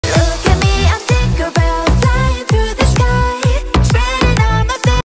Tinker Bell is on the Mushroom! |Ornament
Tinker Bell is on the Mushroom!  It’s a cute ornament at HEB! Its also the perfect place to find Christmas gifts and decor for the holiday season!

(Half of the proceeds will be donated to charity)

It’s great for youtube growth and here’s the link :
Video Makers United: